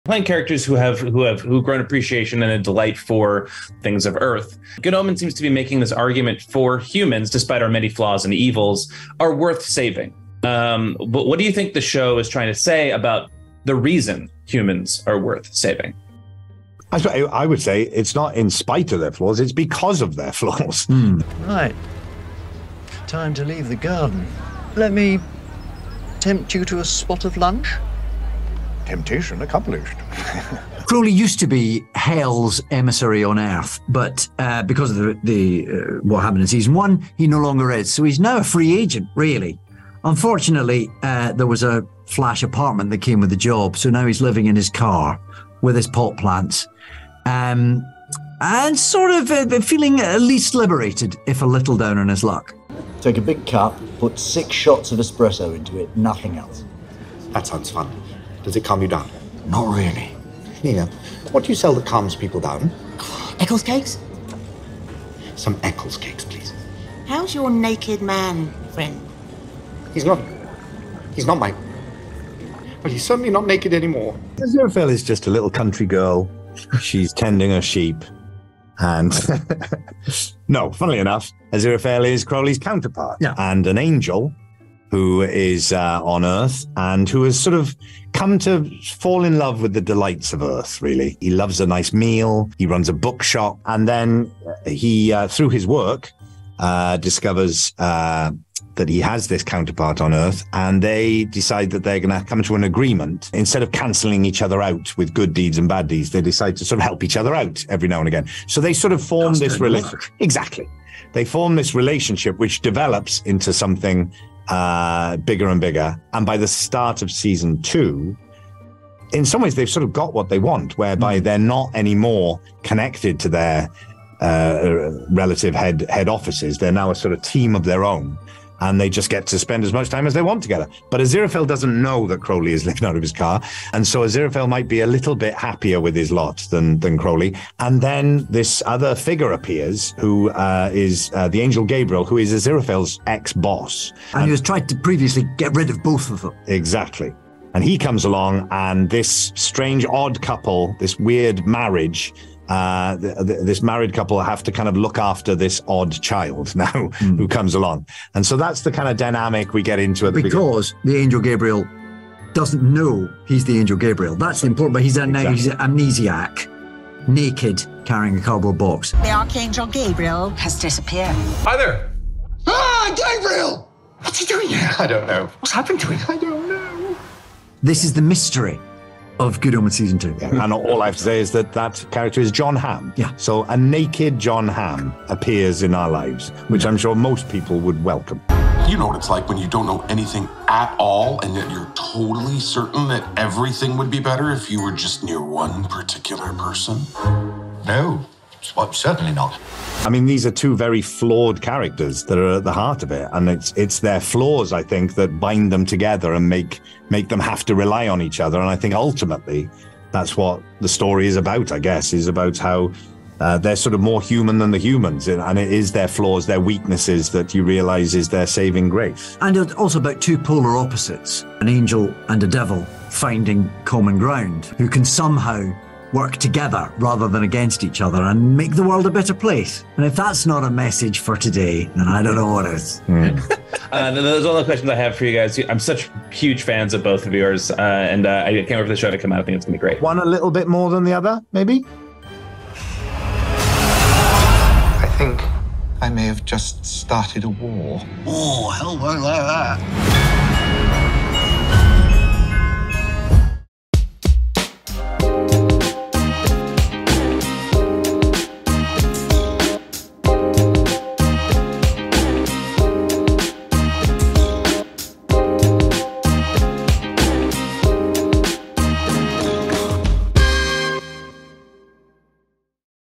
'Good Omens' stars Michael Sheen and David Tennant on why humans are worth saving
Michael Sheen and David Tennant tell us why their Good Omens characters love humans and the delights of Earth.

Good Omens Season 2 is streaming on Prime Video from July 28.

These interviews were conducted before the SAG-AFTRA strike.